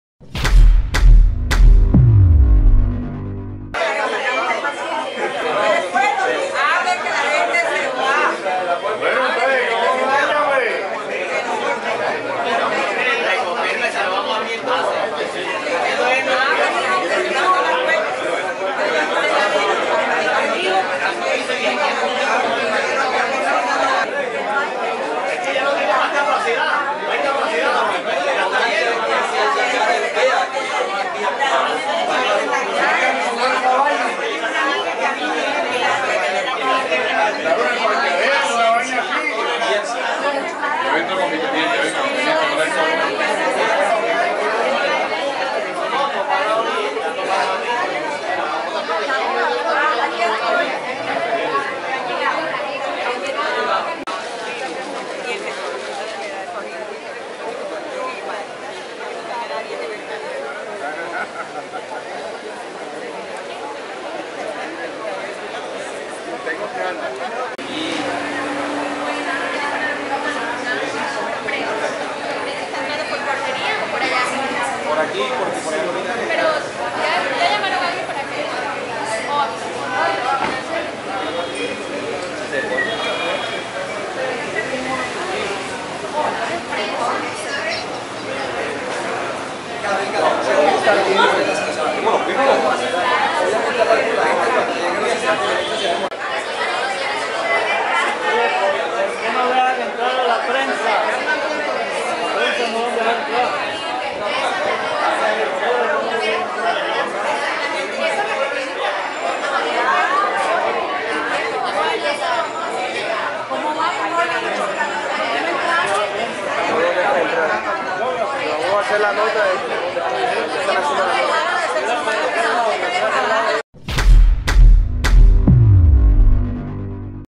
Malestar entre uribistas por traumatismos para ingresar a evento político
Hay descontento entre los uribistas que llegaron al Hotel El Prado con la esperanza de ver al senador Álvaro Uribe y al candidato presidencial del Centro Democrático, Iván Duque, pues a muchos se les negó el ingreso.